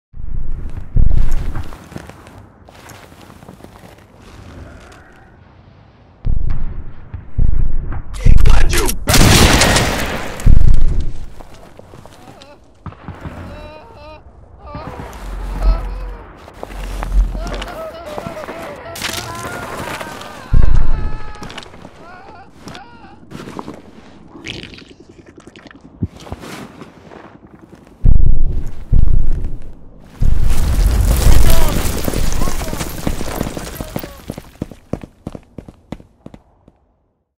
[SFM] Turian (1): SFM Vore Fantasy
Description:

The war still rages on against the "evil" macros. Macro Turian forces have led an army into the town of [REDACTED] to slaughter HPS Personnel and any living micro they come across, using whatever means necessary to complete their mission. HPS and other military forces were sent in to try and contain the chaos that continued for four days straight.

"Our hometown is now completely destroyed, why won't they just leave us alone?" - [REDACTED] Citizen